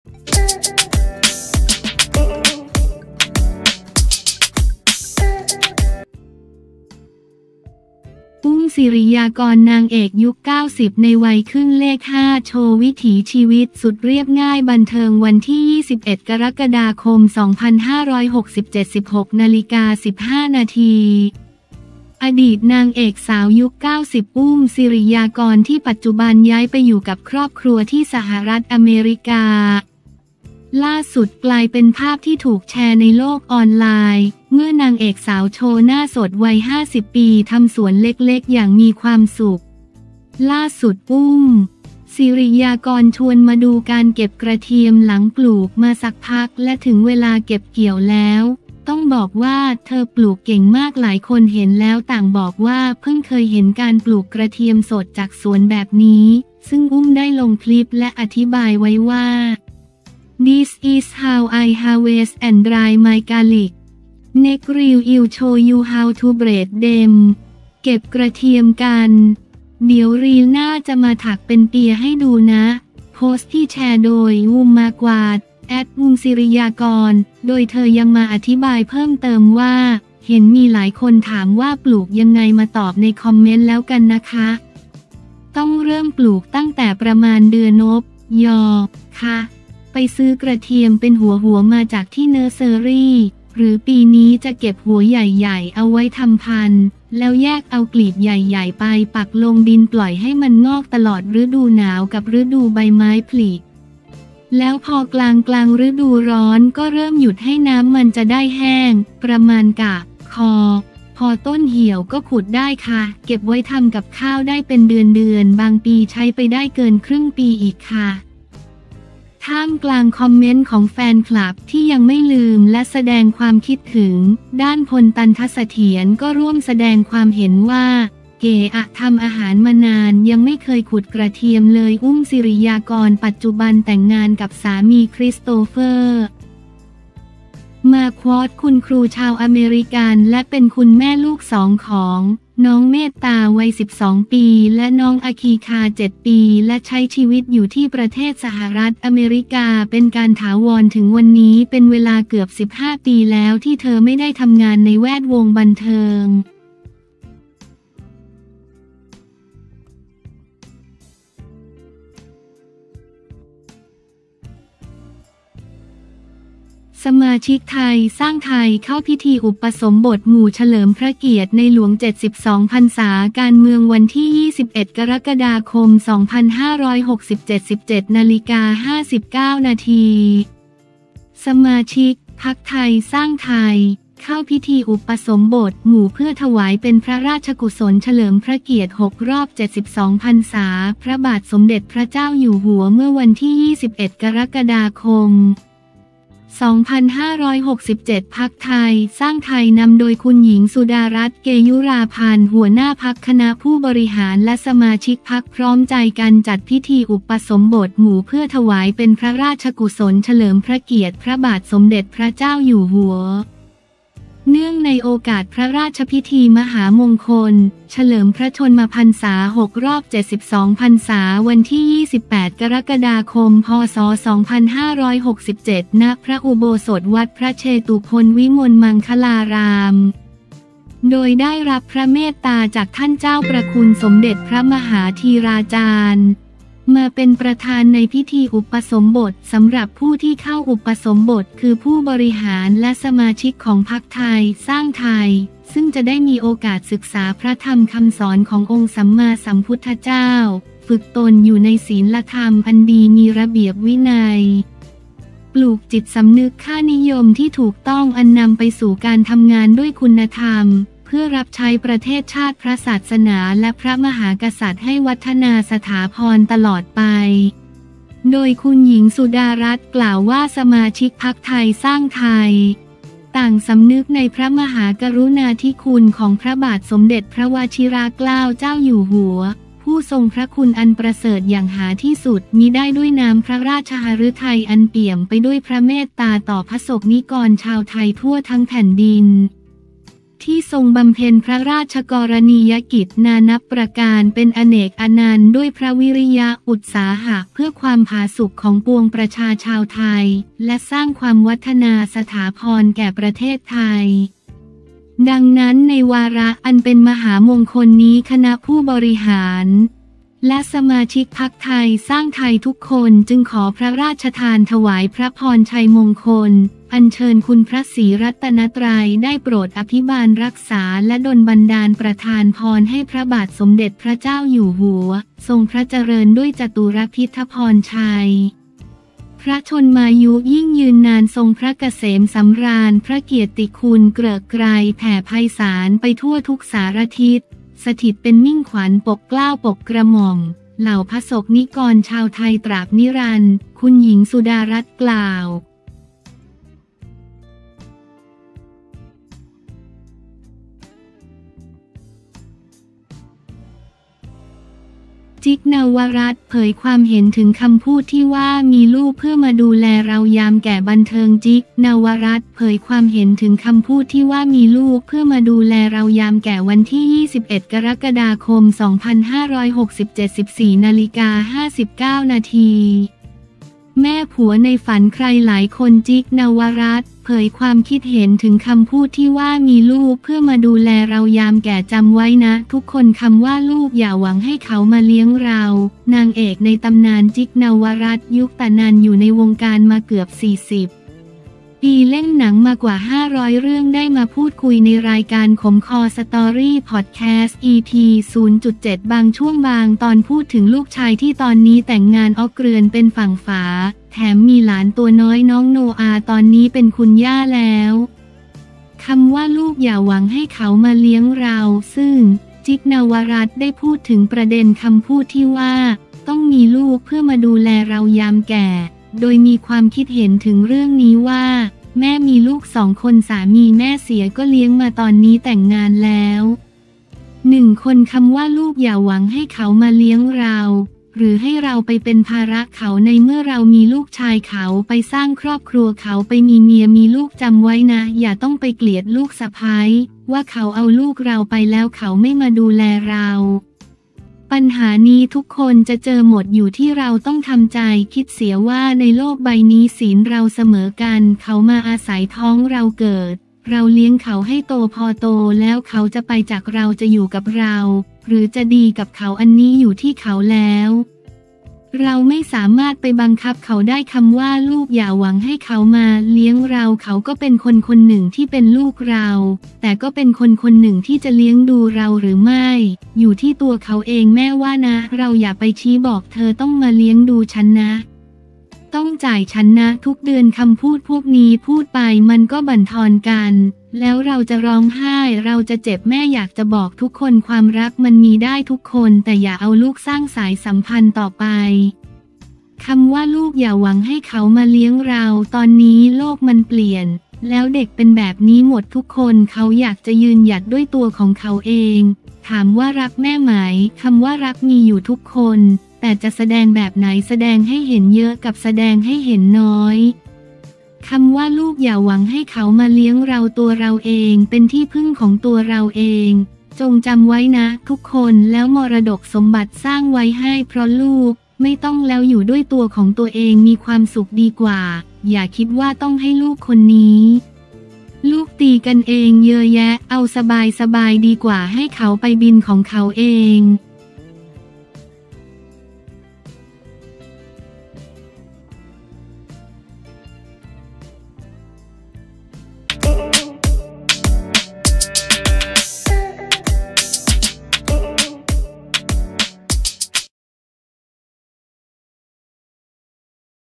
อุ้ม สิริยากร นางเอกยุค90 ในวัยขึ้นเลข 5 โชว์วิถีชีวิตสุดเรียบง่าย
21 ก.ค. 2567
16:15 น.
อดีตนางเอกสาวยุค 90 อุ้ม สิริยากร ที่ปัจจุบันย้ายไปอยู่กับครอบครัวที่สหรัฐอเมริกา ล่าสุด กลายเป็นภาพที่ถูกแชร์ในโลกออนไลน์ เมื่อนางเอกสาวโชว์หน้าสดวัย 50 ปี ทำสวนเล็กๆ อย่างมีความสุข

​ล่าสุด อุ้ม สิริยากร ชวนมาดูการเก็บกระเทียม หลังปลูกมาสักพักและถึงเวลาเก็บเกี่ยวแล้ว ต้องบอกว่าเธอปลูกเก่งมาก หลายคนเห็นแล้วต่างบอกว่าเพิ่งเคยเห็นการปลูกกระเทียมสดจากสวนแบบนี้ ซึ่ง อุ้ม ได้ลงคลิปและอธิบายไว้ว่า “This is how I harvest and dry my garlic! Next reel I’ll show you how to braid them. เก็บกระเทียมกัน! เดี๋ยวรีลหน้าจะมาถักเป็นเปียให้ดูนะ
โพสต์ที่แชร์โดย Oom Marquardt (@oomsiriyakorn)

โดยเธอยังมาอธิบายเพิ่มเติมว่า “เห็นมีหลายคนถามว่าปลูกยังไง มาตอบในคอมเมนต์แล้วกันนะคะ ต้องเริ่มปลูกตั้งแต่ประมาณเดือนพ.ย.ค่ะ ไปซื้อกระเทียมเป็นหัวๆ มาจากที่ nursery (หรือปีนี้จะเก็บหัวใหญ่ๆ เอาไว้ทำพันธุ์) แล้วแยกเอากลีบใหญ่ๆ ไปปักลงดิน ปล่อยให้มันงอกตลอดฤดูหนาวกับฤดูใบไม้ผลิ แล้วพอกลางๆ ฤดูร้อนก็เริ่มหยุดให้น้ำ มันจะได้แห้ง (ประมาณก.ค.) พอต้นเหี่ยว ก็ขุดได้ค่ะ เก็บไว้ทำกับข้าวได้เป็นเดือนๆ บางปีใช้ไปได้เกินครึ่งปีอีกค่ะ
ท่ามกลางคอมเมนต์ของแฟนคลับที่ยังไม่ลืมและแสดงความคิดถึง ด้าน พล ตัณฑเสถียร ก็ร่วมแสดงความเห็นว่า “เก๋อ่ะ ทำอาหารมานาน ยังไม่เคยขุดกระเทียมเลย
อุ้ม สิริยากร ปัจจุบันแต่งงานกับสามี คริสโตเฟอร์ มาร์ควอร์ท คุณครูชาวอเมริกันและเป็นคุณแม่ลูกสองของ น้องเมตตา วัย 12 ปี และ น้องอคีคา 7 ปี และใช้ชีวิตอยู่ที่ประเทศสหรัฐอเมริกาเป็นการถาวรถึงวันนี้เป็นเวลาเกือบ 15 ปีแล้วที่เธอไม่ได้ทำงานในแวดวงบันเทิง